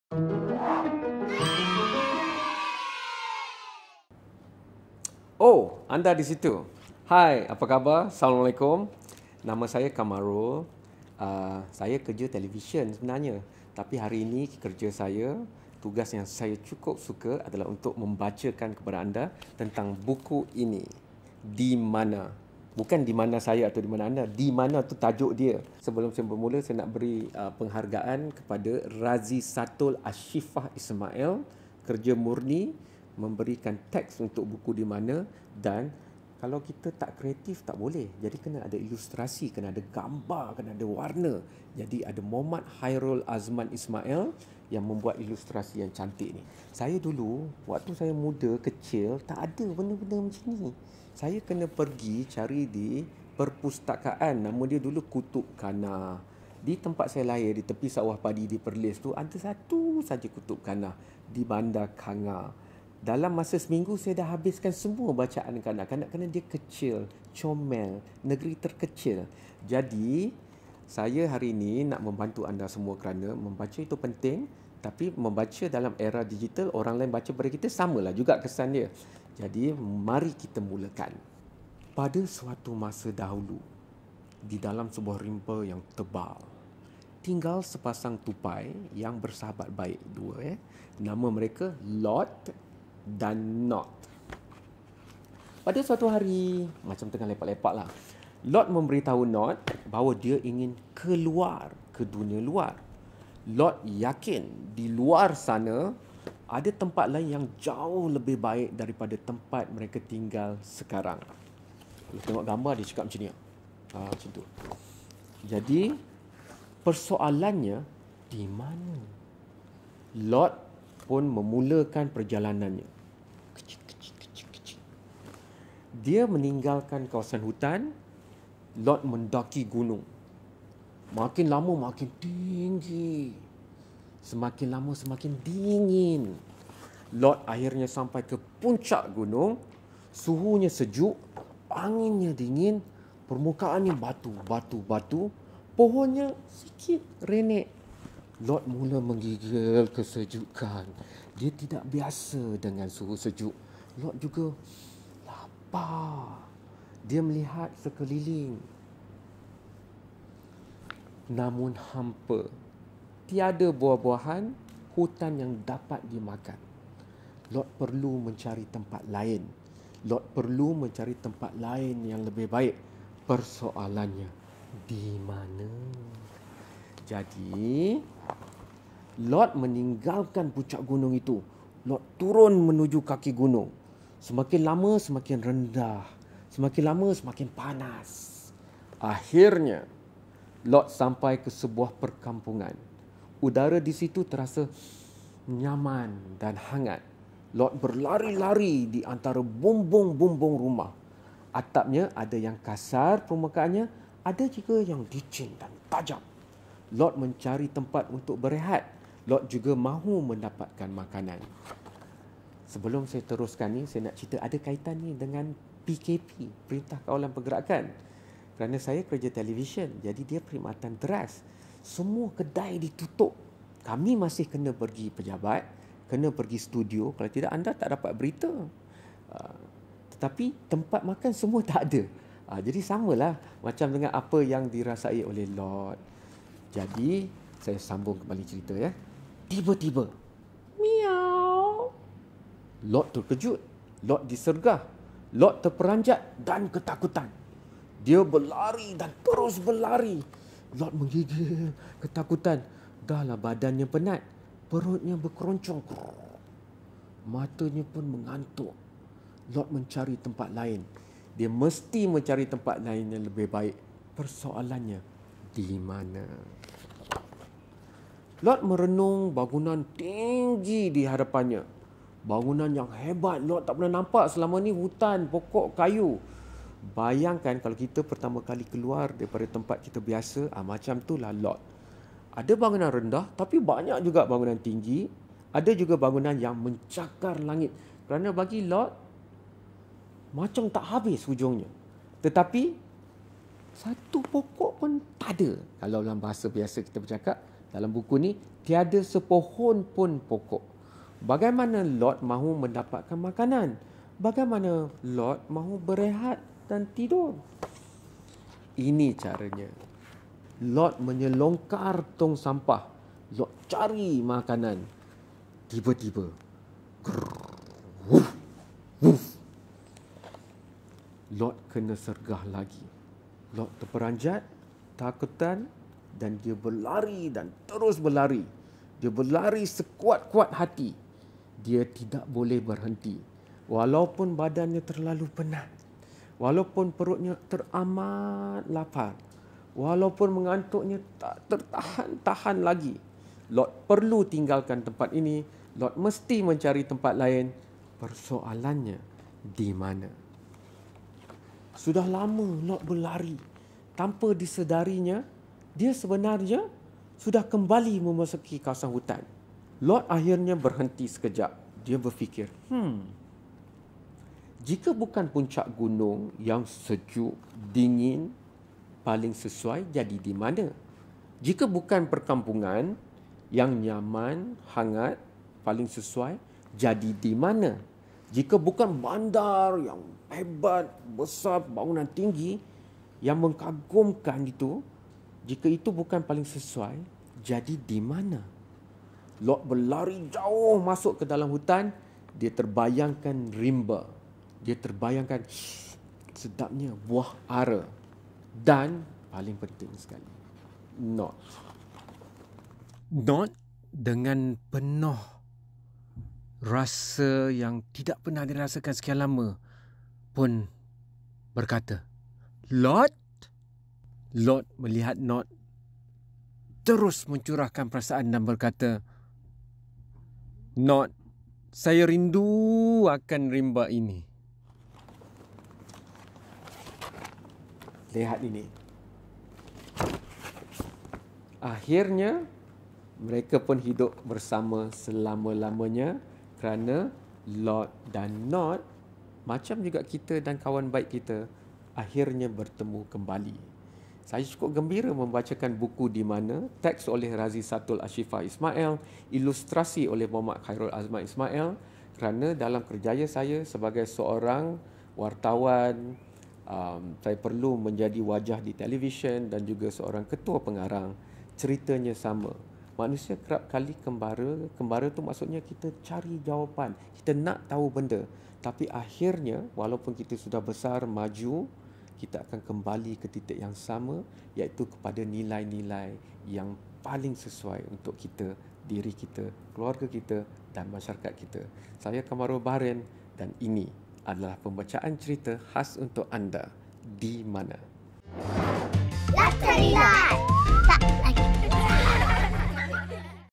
THE STORYTELLER with Kam Bahrin on Lat Tali Lat
TV News Anchor 𝐊𝐚𝐦 𝐁𝐚𝐡𝐫𝐢𝐧 is our next Guest Reader on 𝐓𝐡𝐞 𝐒𝐭𝐨𝐫𝐲𝐭𝐞𝐥𝐥𝐞𝐫.
He will be reading a story in 𝐁𝐚𝐡𝐚𝐬𝐚 𝐌𝐞𝐥𝐚𝐲𝐮 titled 𝐃𝐢 𝐌𝐚𝐧𝐚? a story of a squirrel looking to explore the world.

This book is written by Razisatul Asyifah Ismail and illustrated by Mohd. Khairul Azman Ismail.